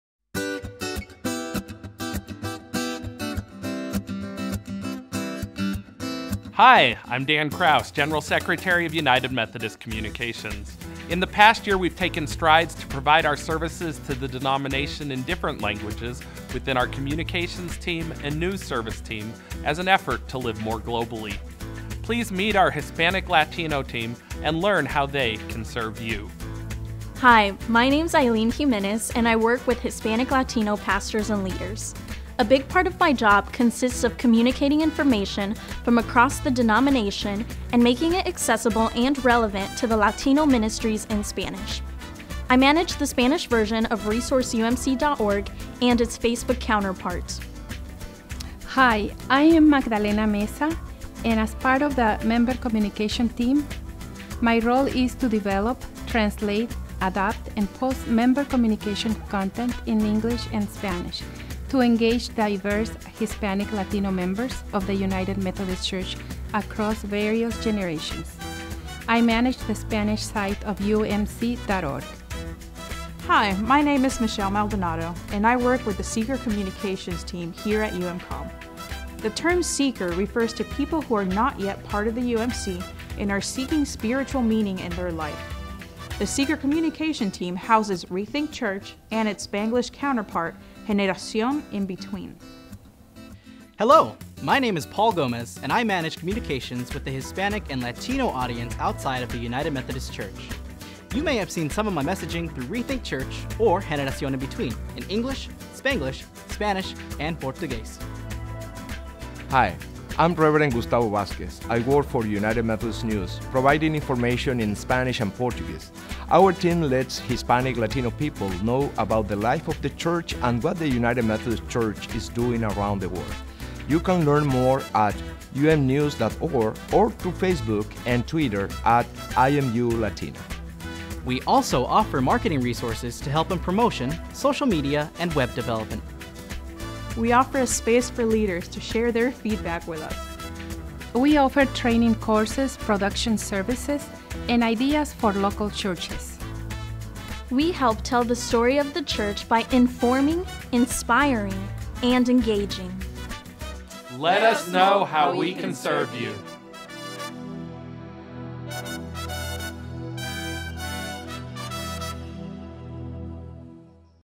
Meet the UMCom Hispanic/Latino Staff
United Methodist Communications has been making an intentional effort to provide resources and services to the denomination in different languages in an effort to live more globally.

Meet our Hispanic/Latino team and learn how they can serve you.